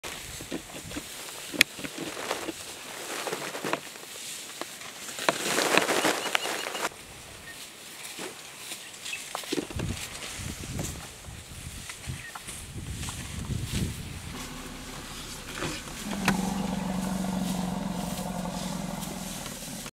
Shy baby elephant
A shy baby elephant was spotted in the Kruger National Park. After patiently waiting for a few minutes, the little elephant emerged from behind his mother to greet us.